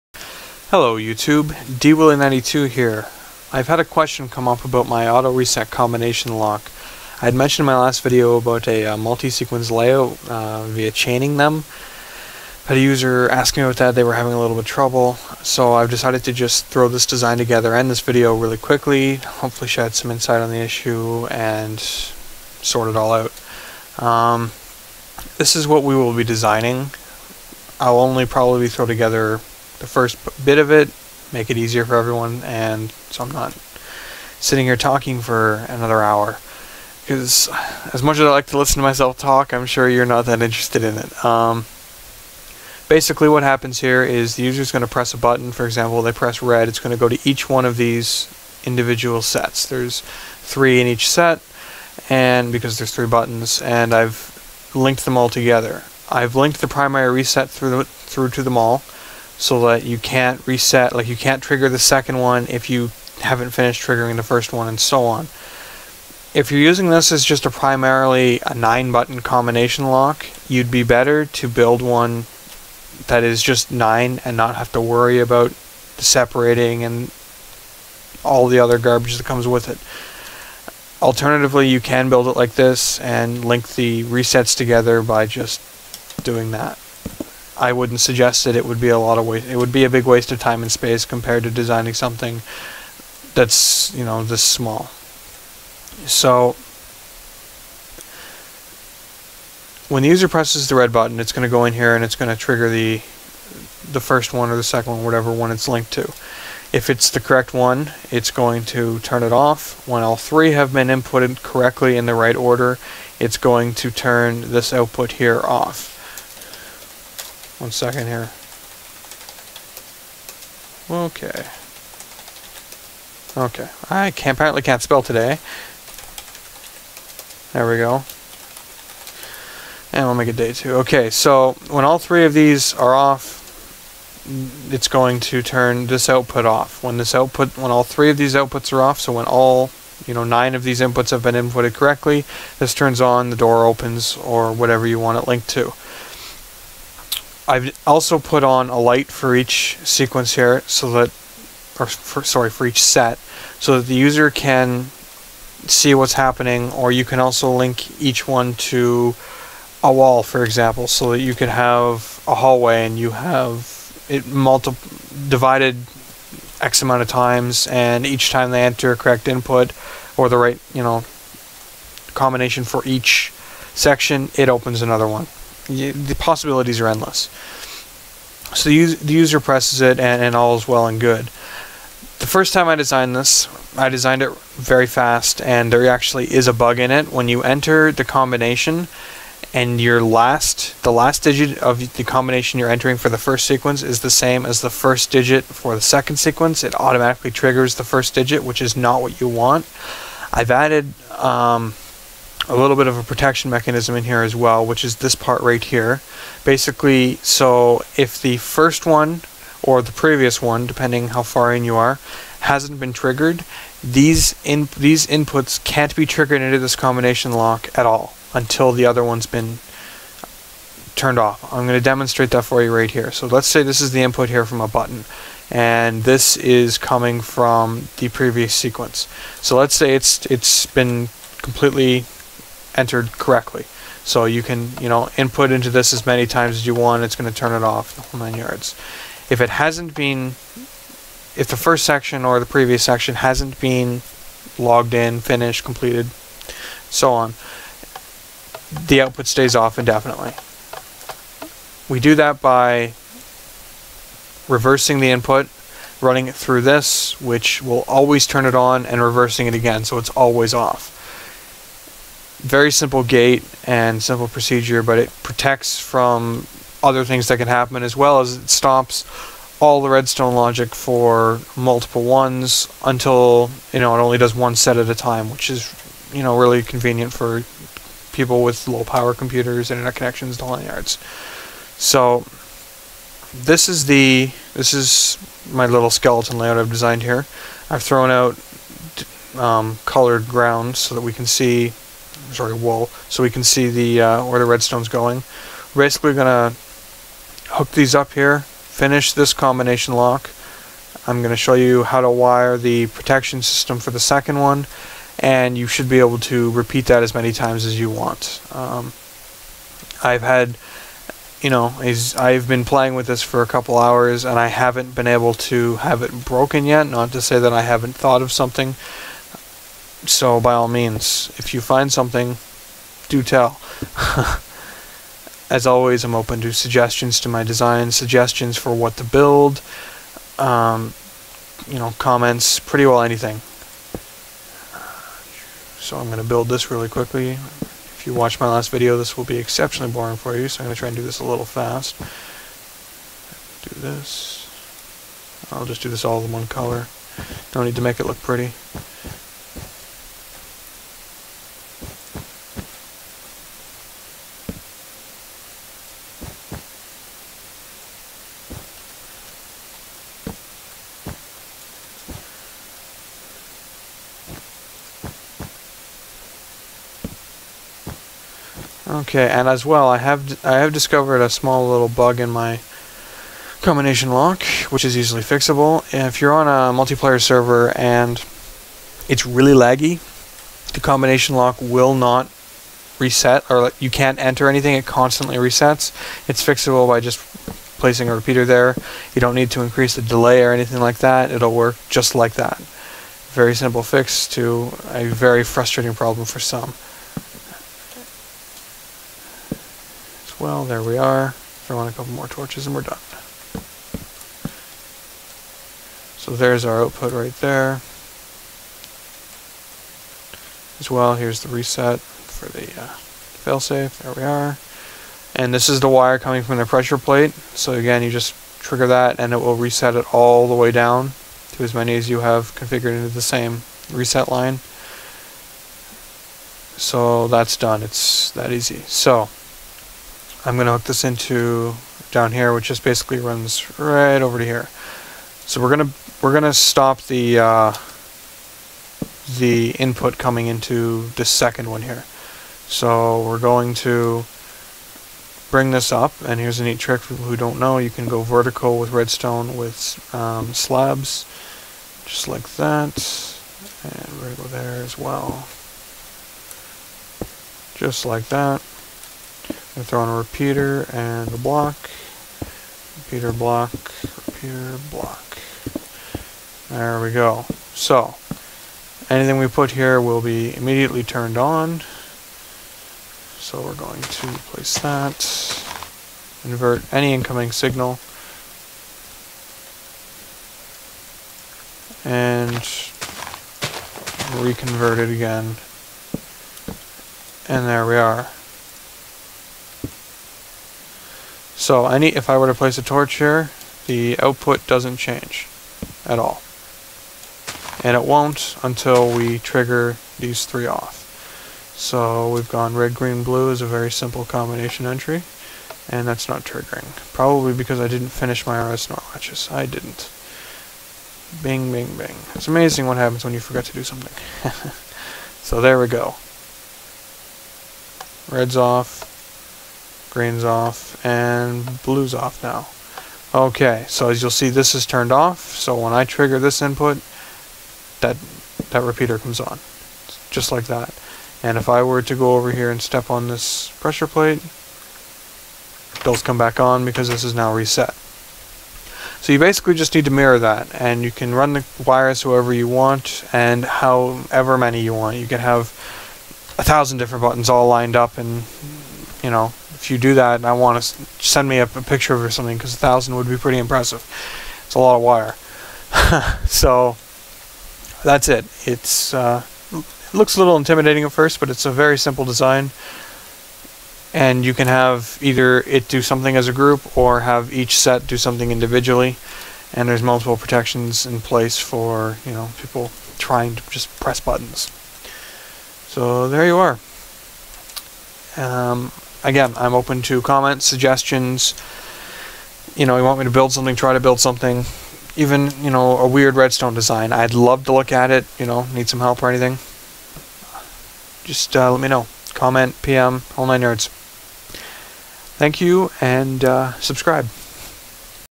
Chaining Combination Locks
A quick How-To on chaining combination locks, a followup to my other video "Auto-Reset Combination Lock"